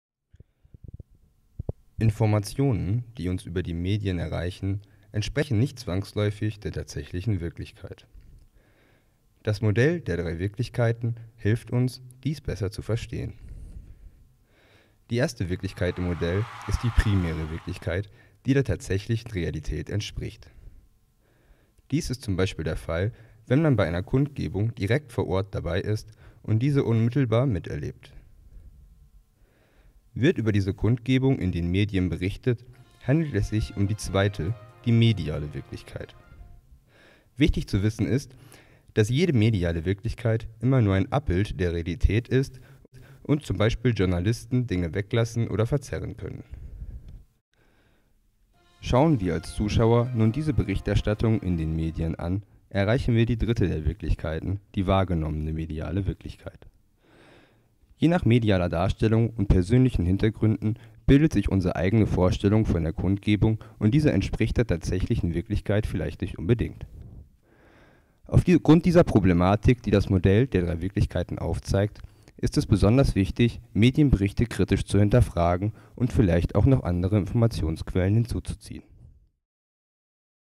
Modell der drei Wirklichkeiten
Zwischen dem, was wirklich passiert und dem, was wir durch die mediale Berichterstattung dann darüber denken, sind bei genauerer Betrachtung deutliche Unterschiede festzustellen.
Warum das so ist, beschreibt dieser feine Erklärclip.